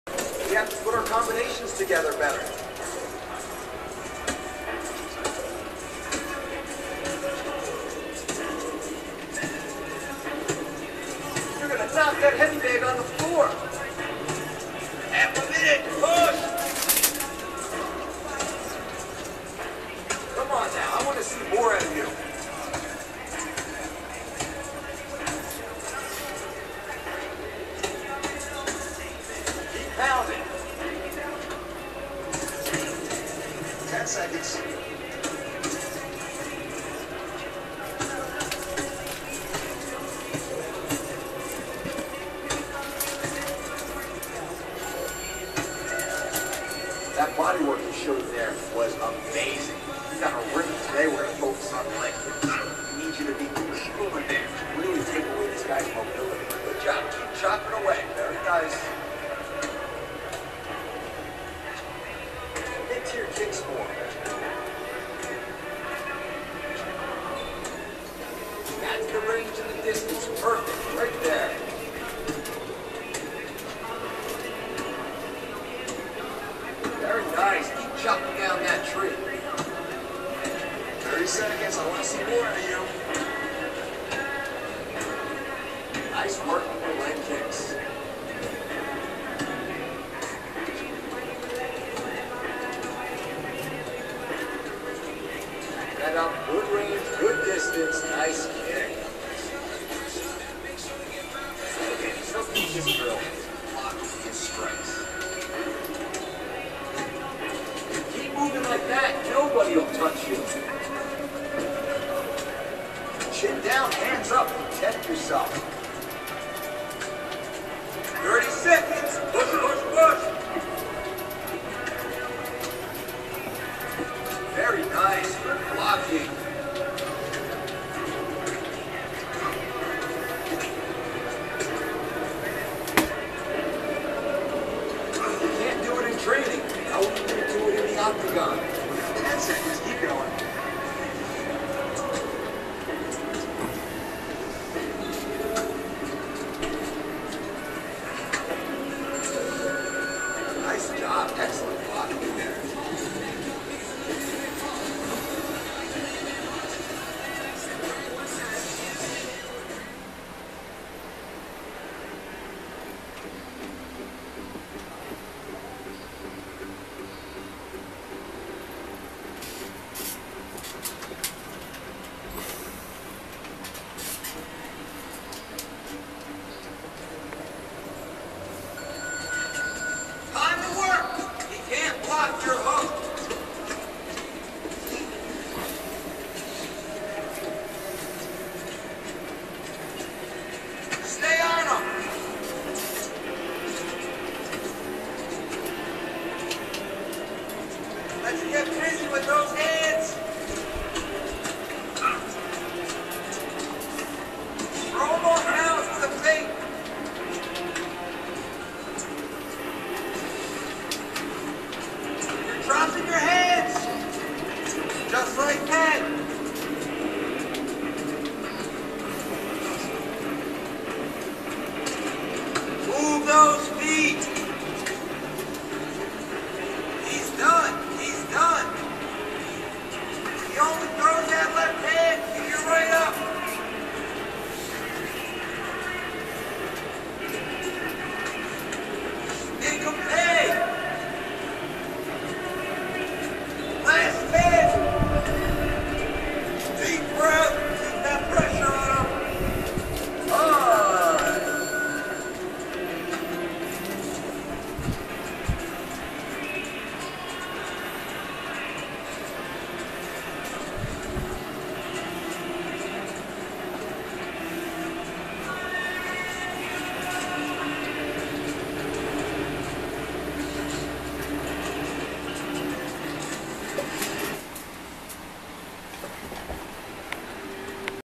CESAR MASKS PLUS OTHERS
Description masks in bedroom feat. Cesar, some rudel West  Germany, plus others